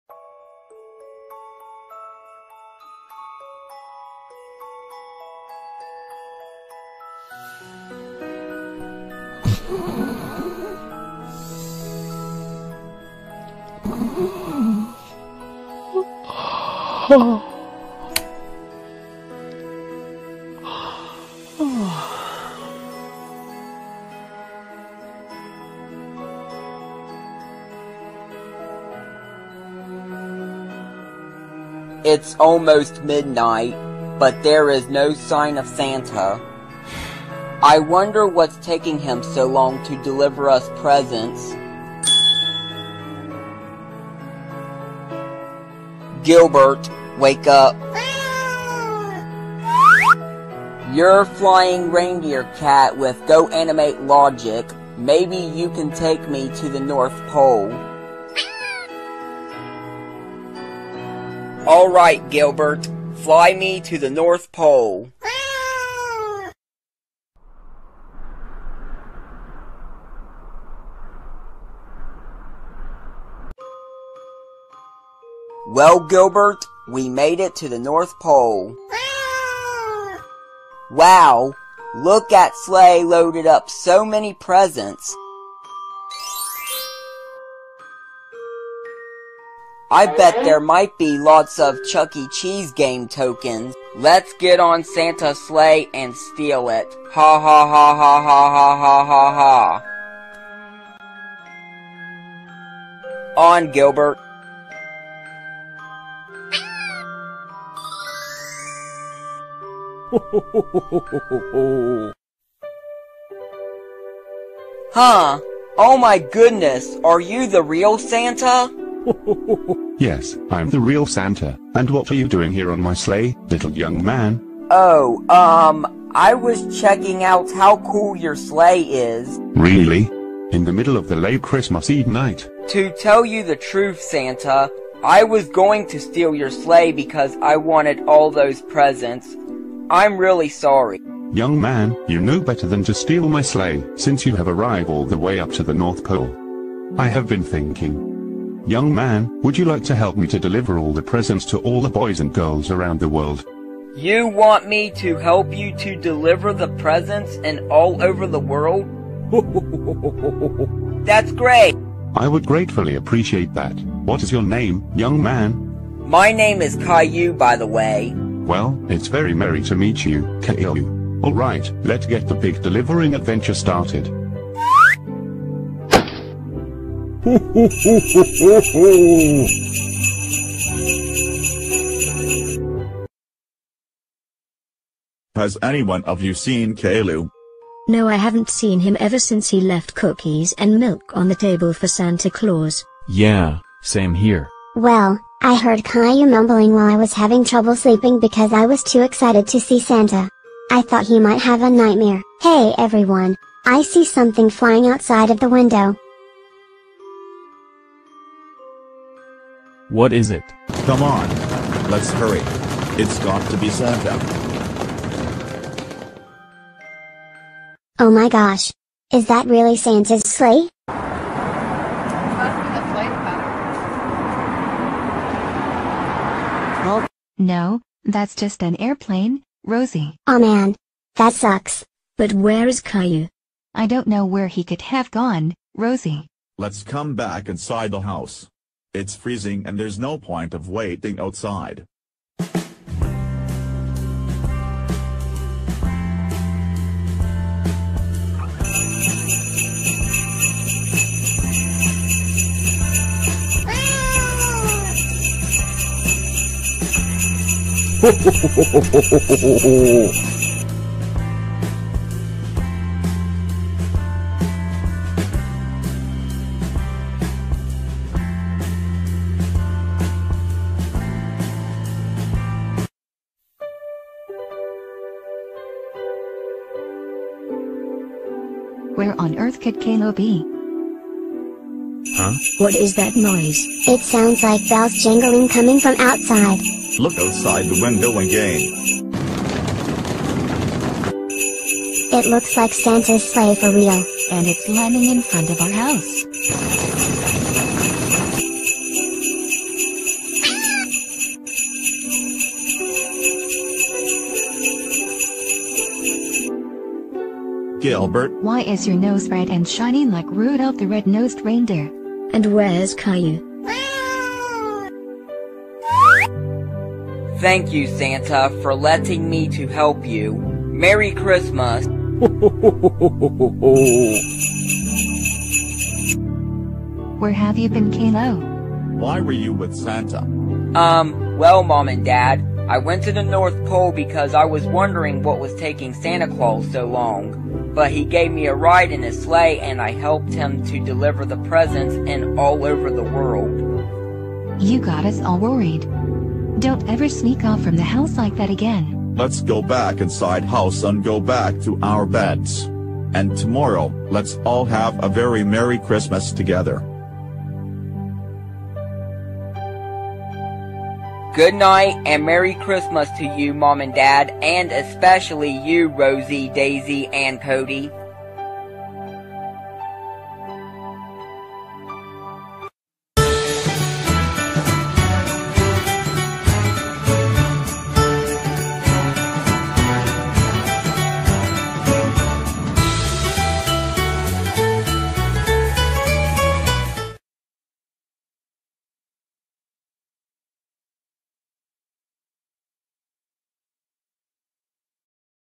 Caillou Gets Ungrounded Episode 1: Caillou Visits The North Pole
Give Credit to African Vulture Caillou (Rod Westheider) helps Santa deliver the presents for Christmas